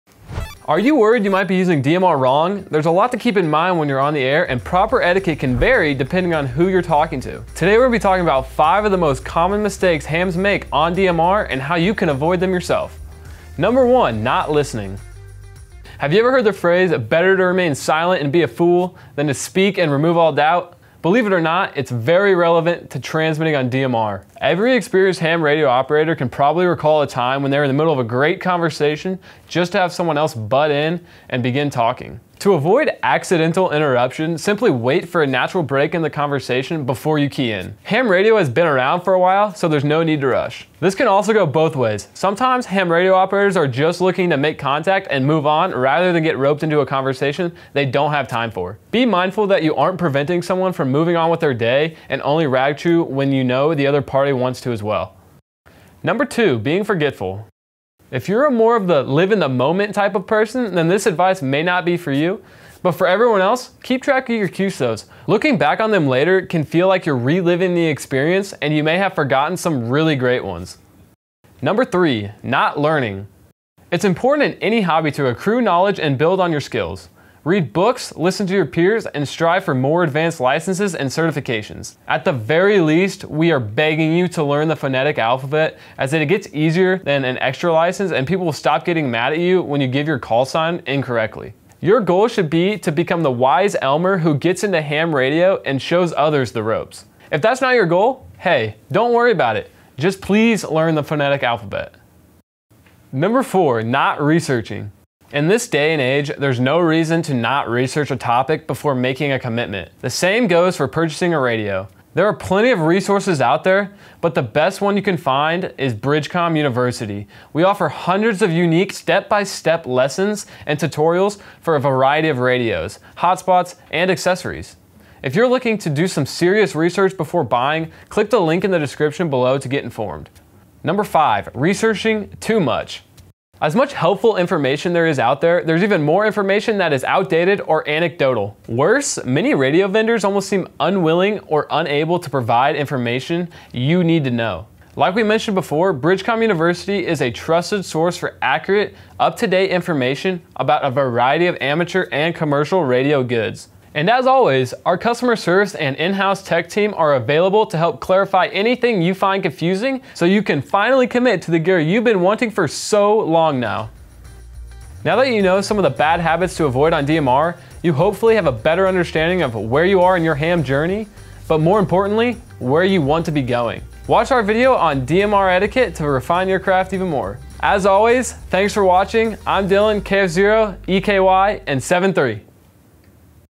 Common Mistakes Hams Make on DMR Radio
Are you making mistakes when you're on the air with ham radio? Our new video goes over some of the most common mistakes hams make every day on the air. Watch it now to make sure you aren't making those mistakes!

0:00 Intro
0:13 Not Listening
0:55 Being Forgetful
1:11 Not Learning
1:42 Not Researching
2:11 Researching Too Much
2:44 Next Steps